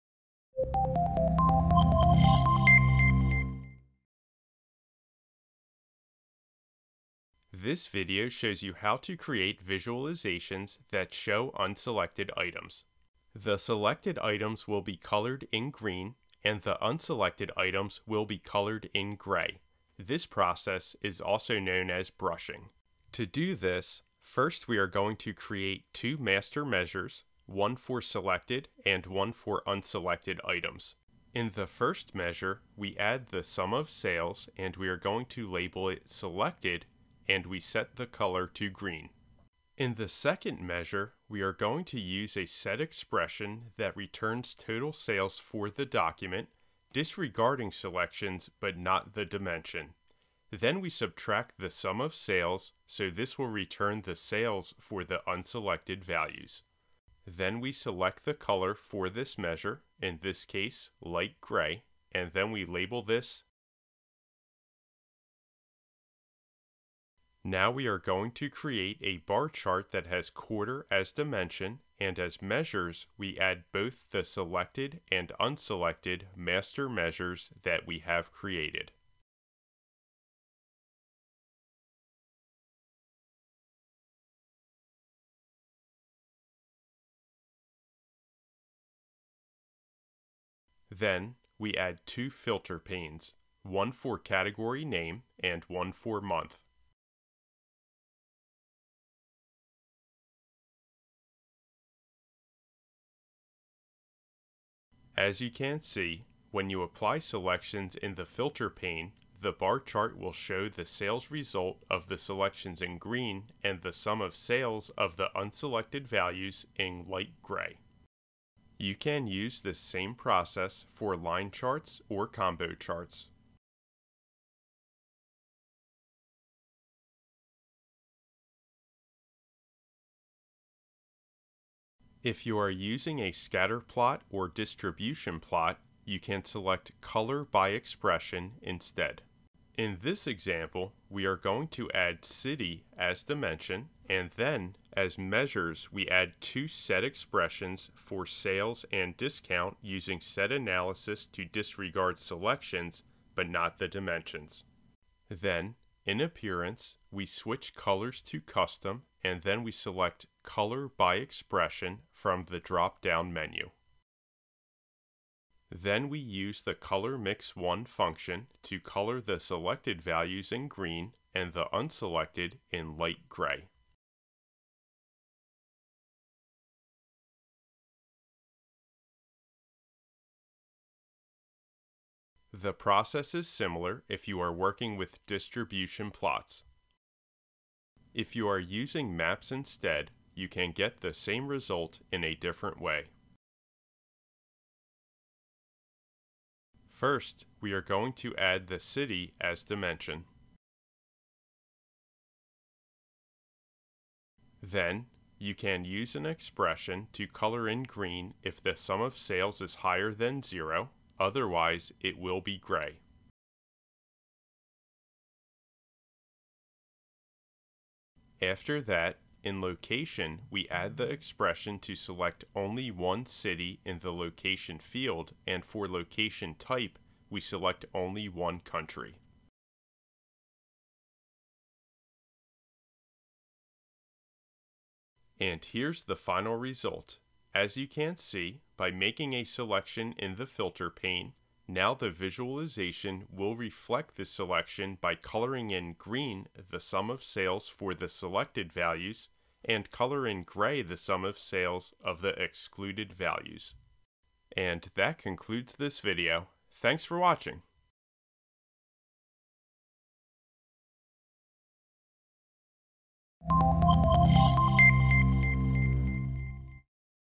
Brushing in Qlik Sense - Qlik Tips and Tricks
In this video, learn how to create visualizations in Qlik Sense that highlight selected and unselected items, also known as brushing. We’ll guide you through setting up master measures for selected (green) and unselected (gray) items and applying them to various charts.

First, we'll create master measures for selected and unselected sales. Next, we’ll use these measures in a bar chart with quarters as the dimension and add filter panes for category and month. You’ll see how selections dynamically update the chart.

We'll also cover applying this technique to line charts, combo charts, scatter plots, distribution plots, and maps. For scatter plots and distribution plots, we’ll use the "color by expression" feature, and for maps, we'll use expressions to color locations based on sales.

By the end of this video, you'll be able to create dynamic visualizations that differentiate between selected and unselected items, enhancing your data analysis in Qlik Sense.

00:00:07 - Introduction
00:00:22 - Creating master measures
00:00:47 - Setting up the bar chart
00:01:07 - Adding filter panes
00:01:35 - Brushing in line and combo charts
00:01:55 - Color by expression in scatter and distribution plots
00:02:28 - Setting up maps